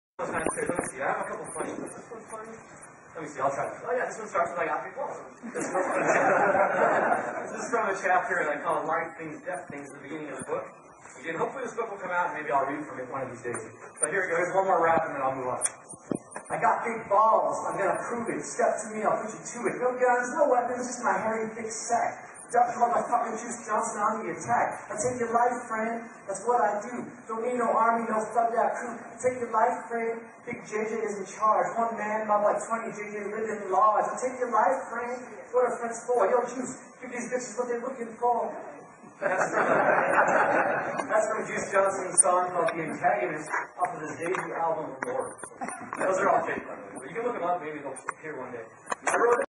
Mike Calvillo Raps
Author and friend, Mike Calvillo raps from his upcoming book